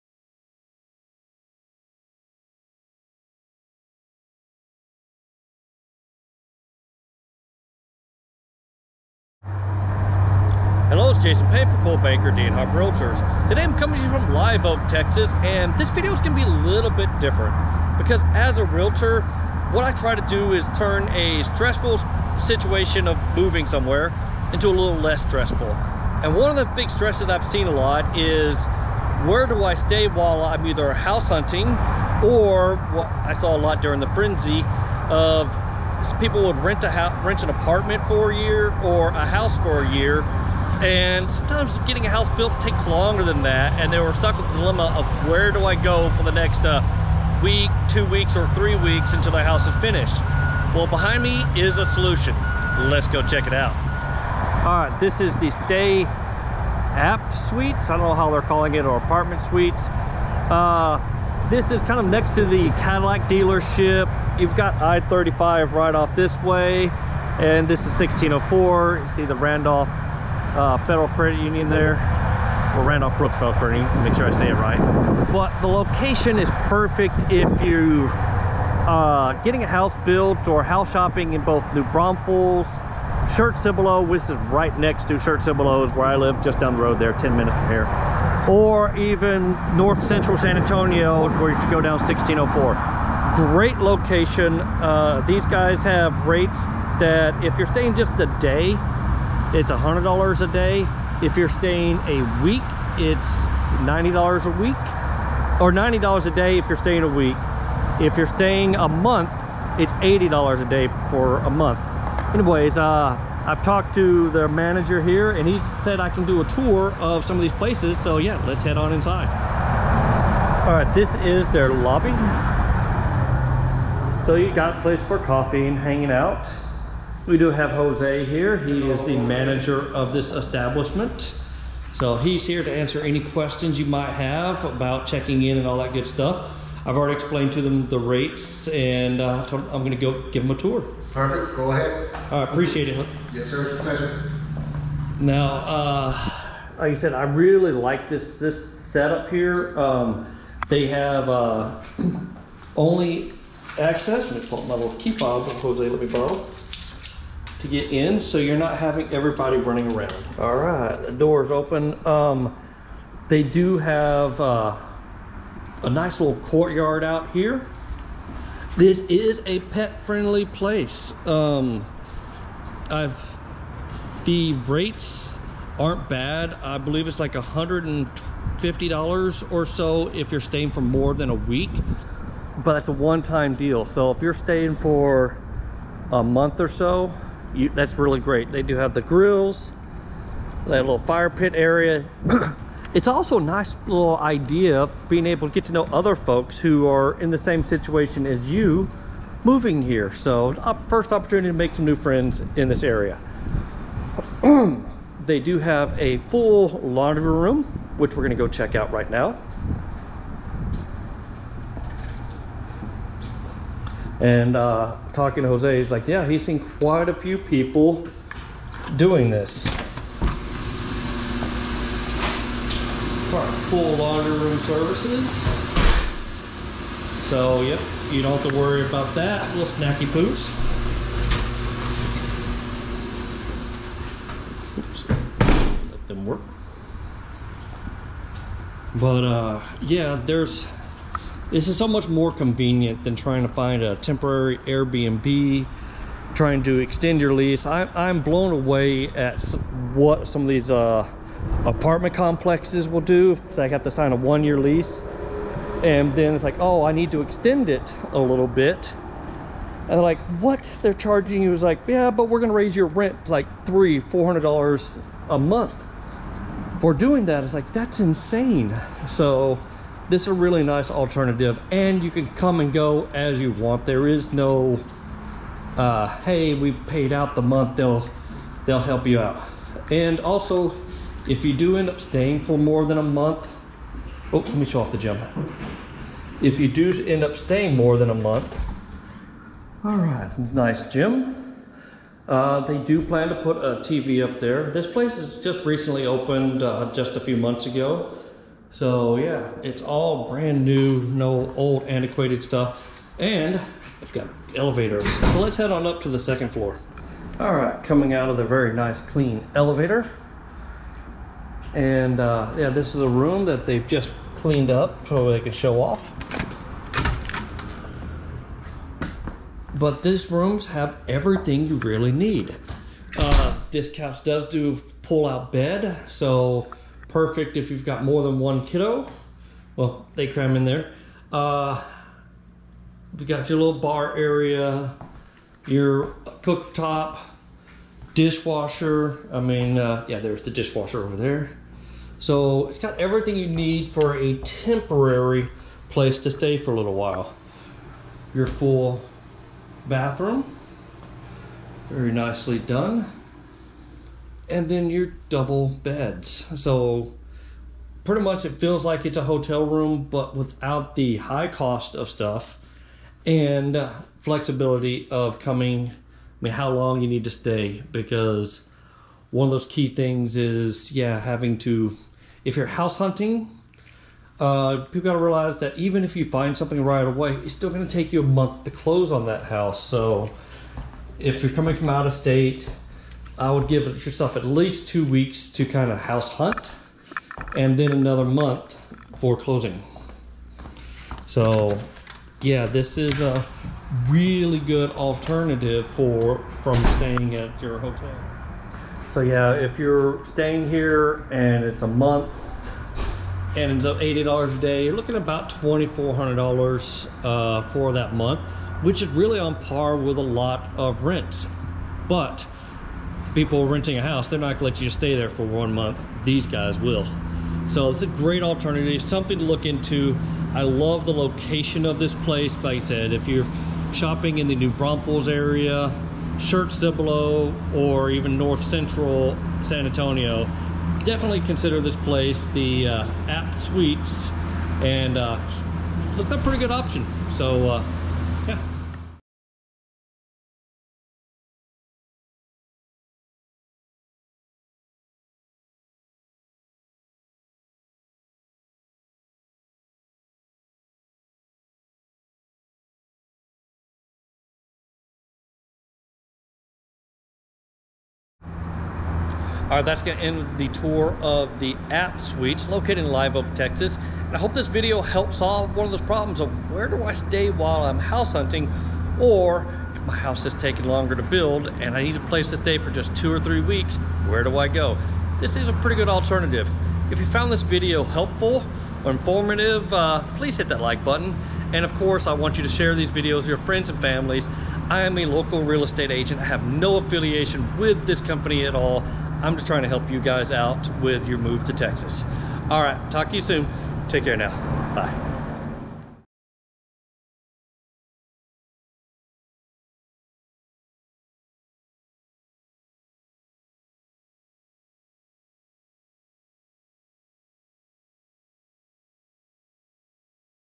Temporary Place to Stay: Stay Apt Suites, NE San Antonio Tx,
This place is perfect if you need to only be somewhere for a few days, weeks or months. Easy access to Randolph AFB, Schertz, Cibolo, North Central San Antonio and even New Braunfels Tx. I am a local Realtor that specializes in helping people relocate the this part of Texas feel free to text me a or click this link to schedule a Zoom meeting